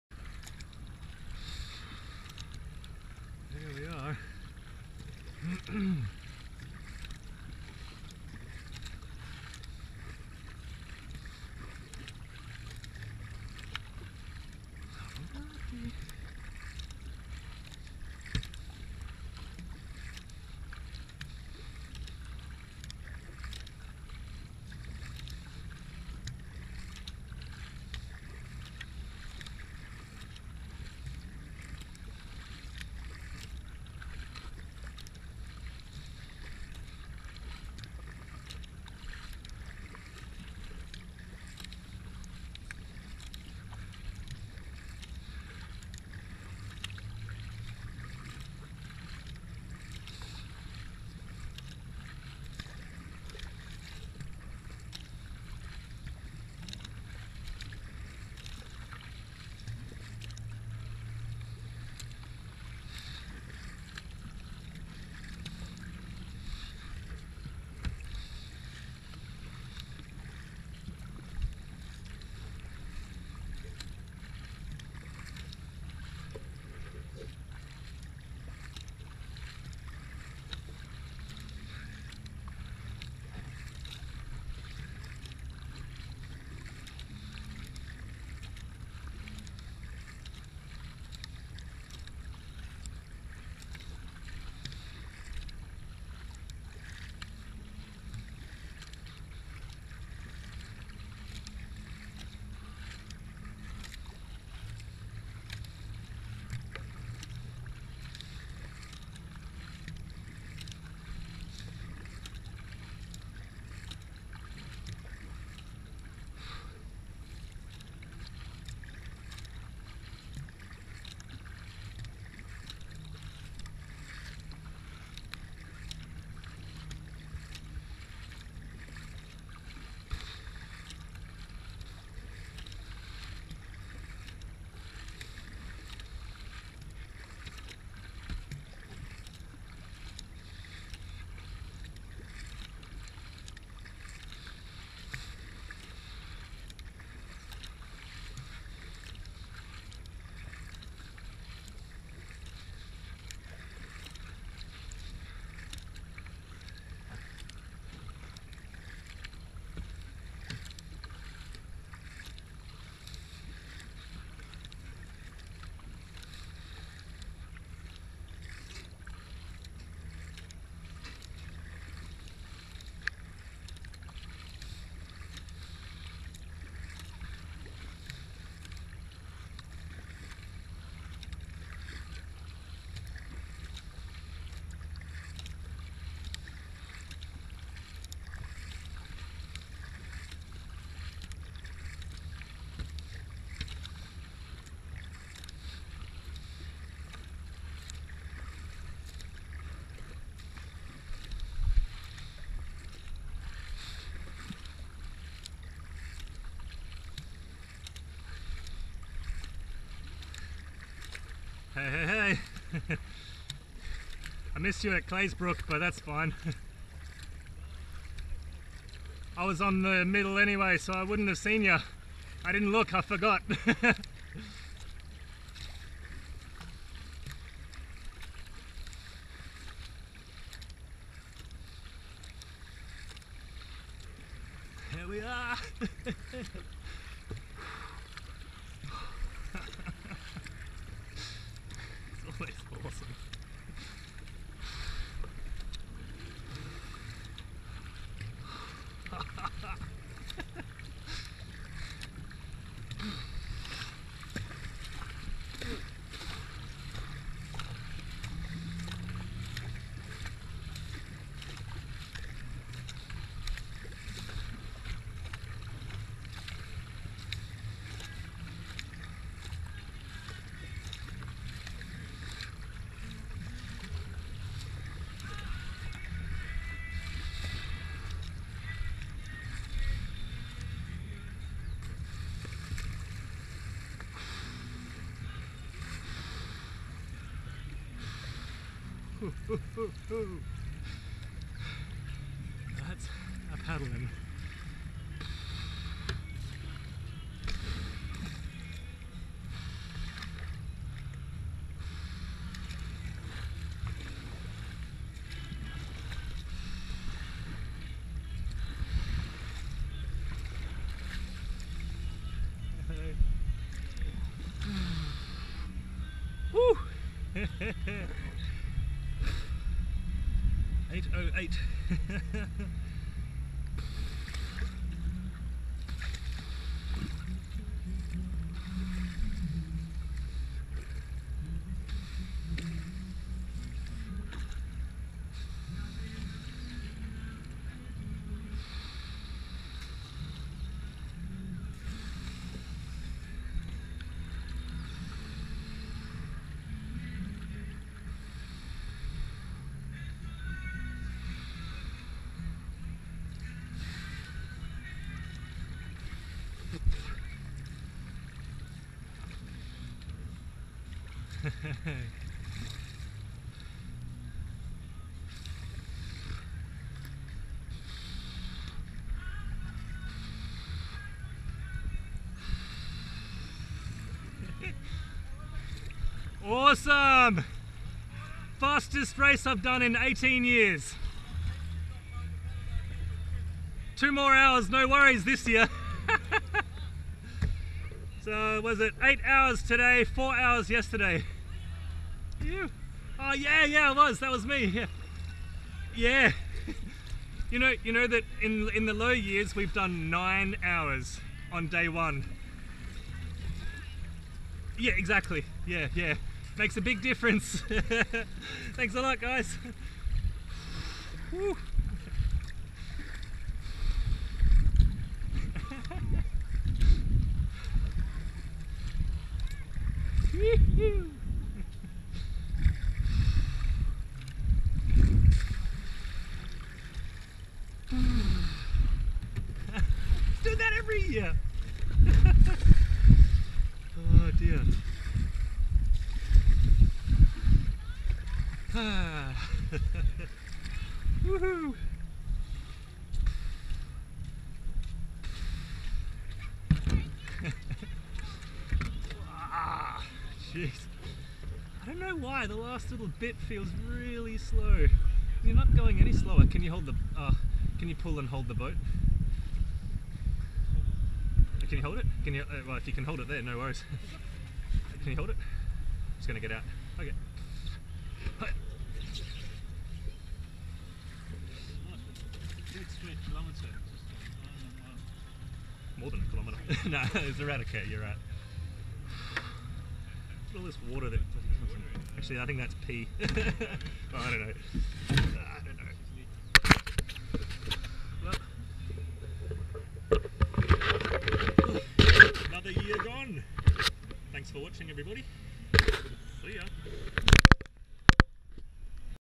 Avon Descent 2021 - 44 Day2 Elizabeth Quay FINISH!!
Helmet Camera footage of the 2021 Avon Descent. This one shows my final stretch past Barrack Street Jetty and into Elizabeth Quay. Fastest time I've ever done by a long way.

I vote for the race to be extended to finish here.
This would be a massive boost to the Quay, would give it a river related purpose other than ferries, and would raise the profile of the Avon Descent as everyone in the City would know it's on and be able to see the finish without going out to Bayswater.
Yes its around 2 hours further, but with management of the cut-off times and possible mandatory lights past 5pm (cheap and common that most of us already have), it would not be any harder to organise; there are already some quite large evening paddling events on the Swan River..
Powerboats would have all finished well before dark so would be no different for them, in fact they could finish the race at Elizabeth Quay then power back to their own clubhouse, 1km back upstream at Burswood!

The water inside the Quay smells fishy again, need to clean it guys :P

Another year gone, hope you all enjoyed watching, leave some comments or suggestions if you'd like, see you on the next exciting adventure!

The predicted water level at Walyunga today was about 1.8m but it felt more like 2.3m